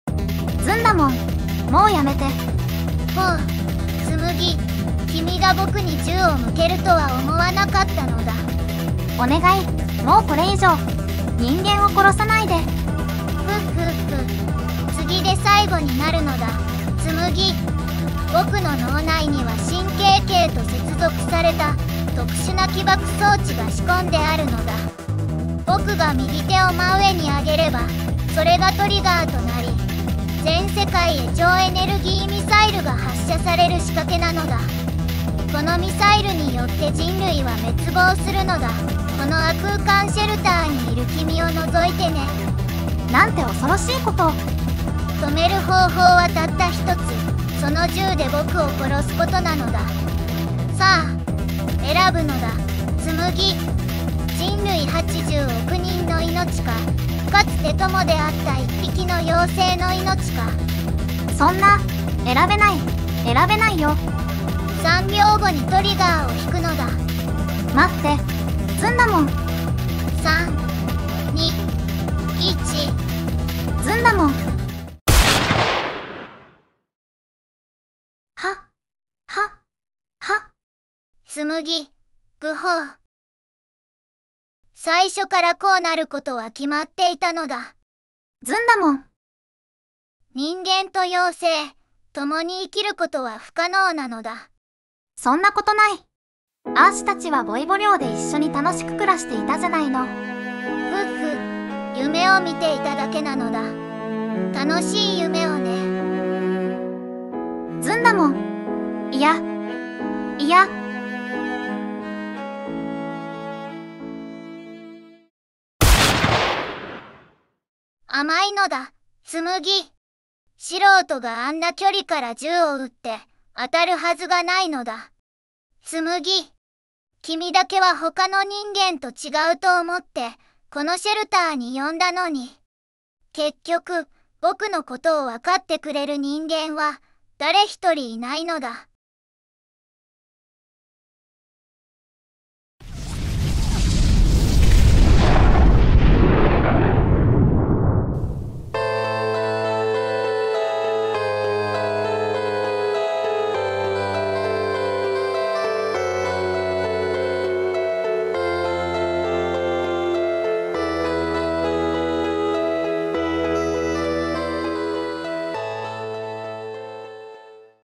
人類VSずんだもん！生き残るのはどっちだ！？【VOICEVOX劇場】
めたんも、はうも、そらも、みんな殺された…。人類への憎悪から殺戮を繰り返すずんだもんを止めるため、春日部つむぎは意を決してずんだもんに迫る。

素材：
坂本アヒル　さん

ずんずんずるるん　さん